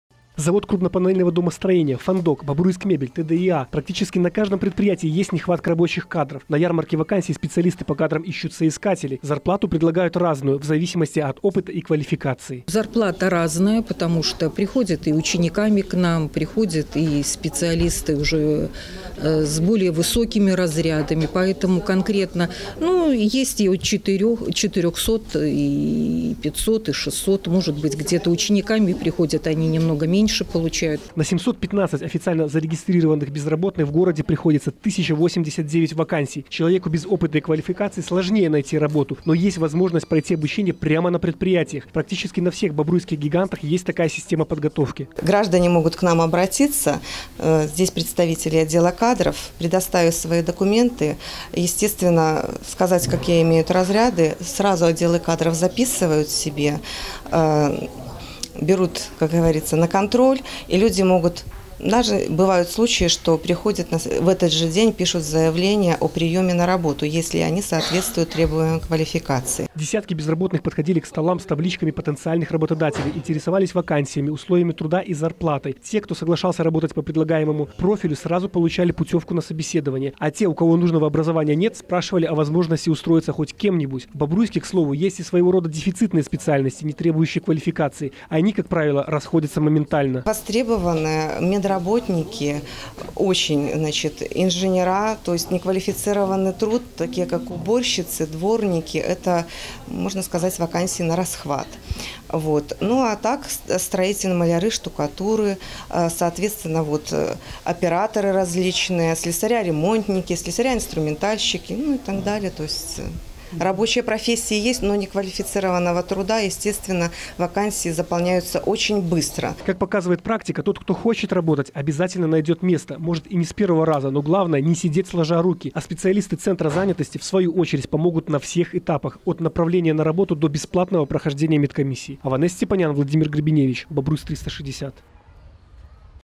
Ярмарка вакансий
Представители предприятий города общались с желающими трудоустроиться. Для некоторых из них собеседование стало прямой дорогой к рабочему месту. Но действительно ли все профессии важны?